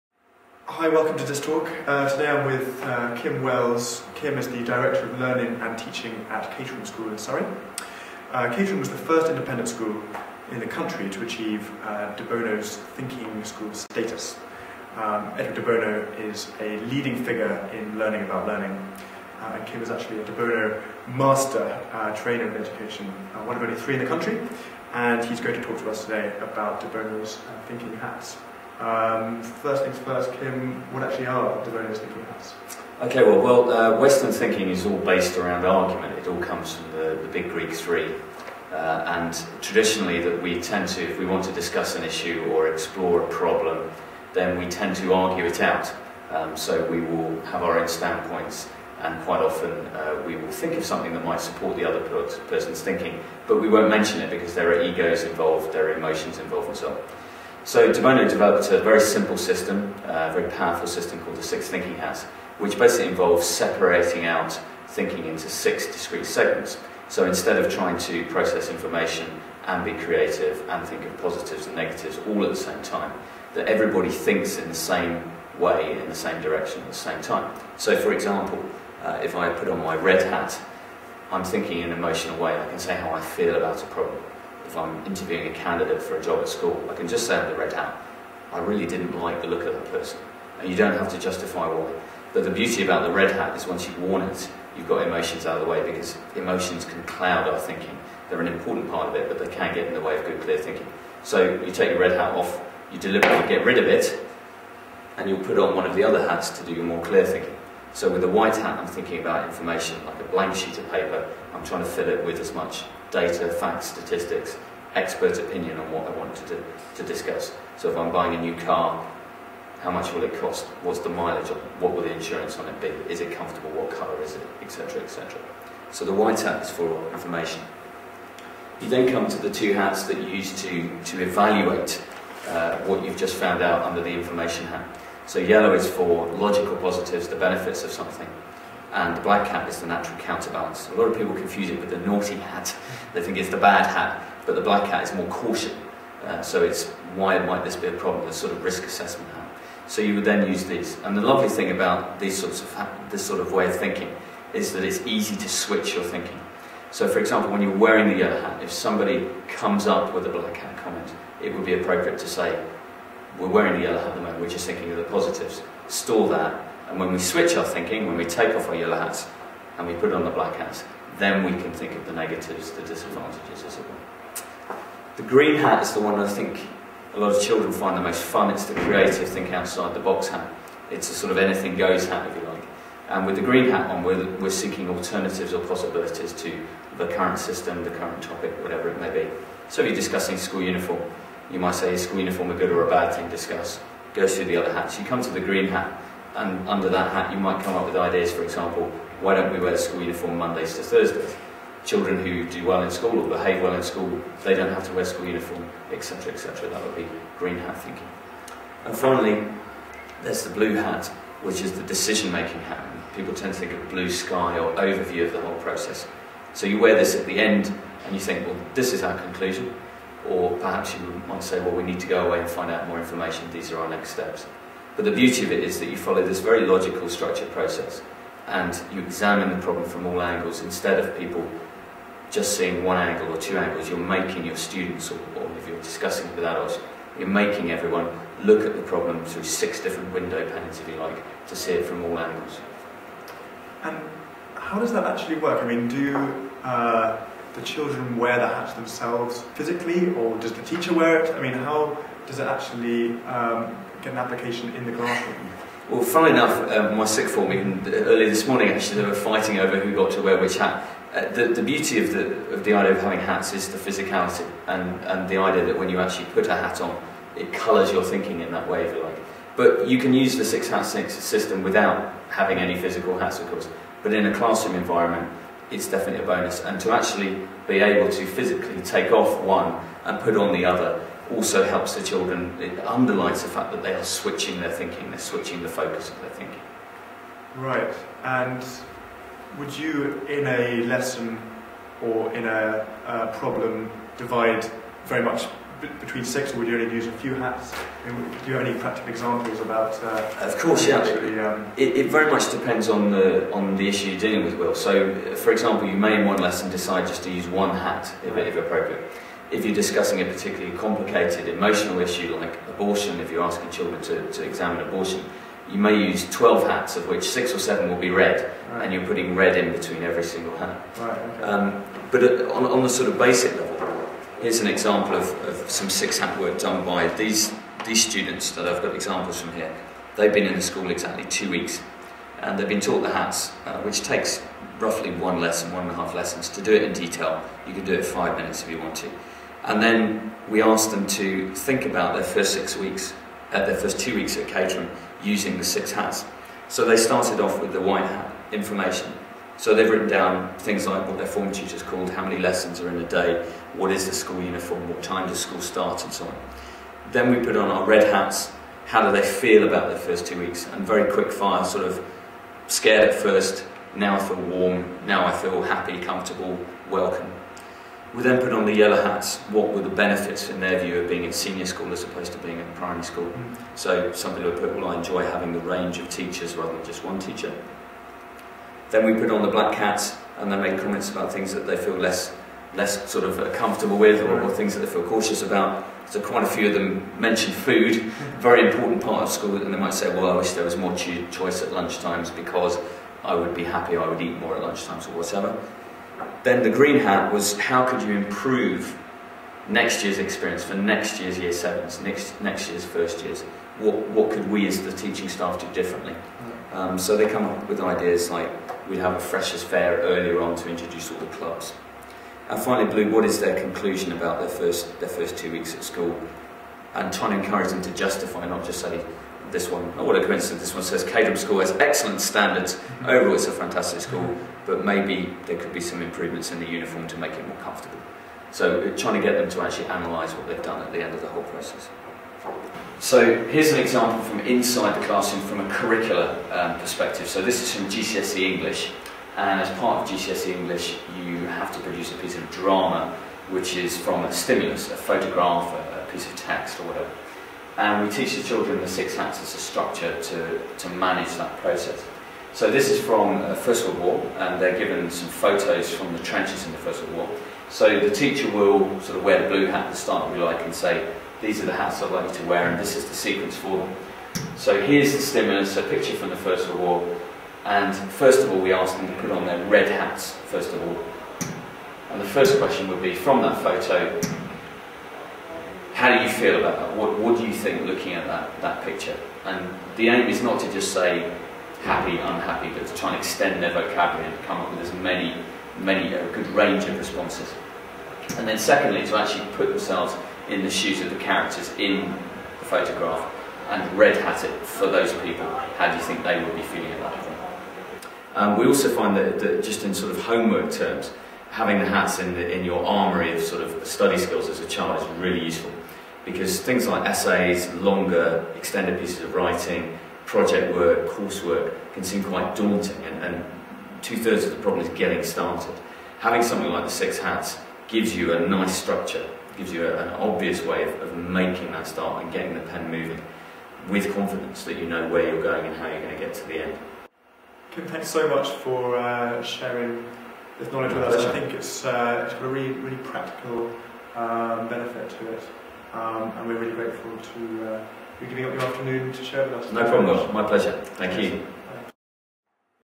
de Bono Thinking Hats
What are Edward de Bono's six thinking hats? Kim Wells, one of three de Bono master trainers in education, describes to dysTalk what de Bono's thinking hats are and how they can be used as a learning and thinking strategy.